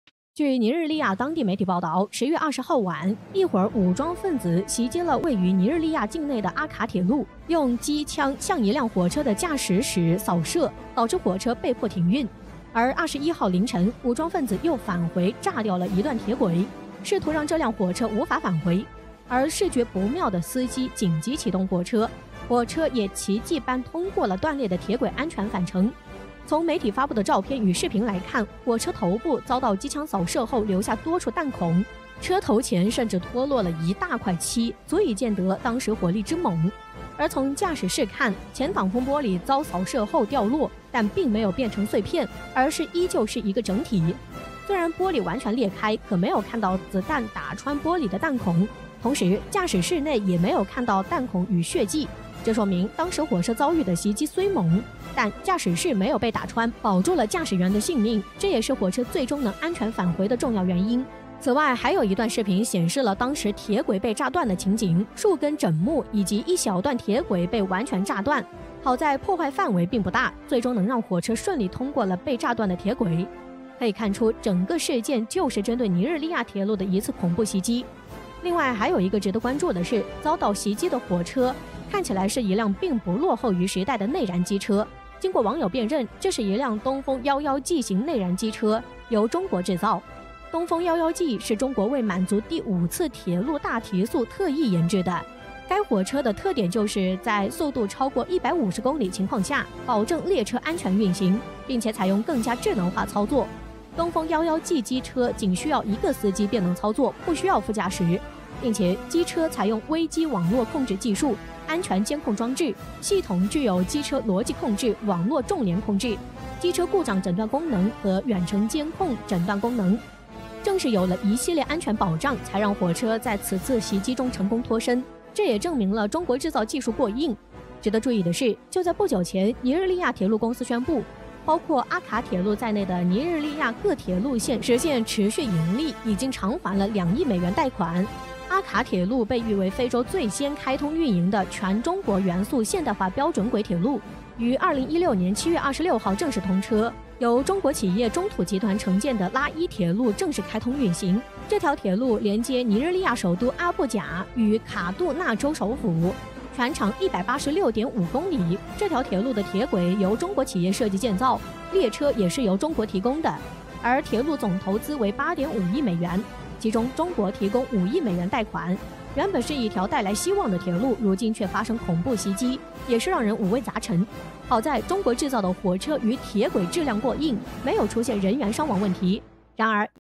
突发，中企承建铁路被炸！中国火车有多“坚强”？挡住机枪扫射，靠着断裂的铁轨安全返程，西方却忙着泼脏水
微信号：
wangyang14121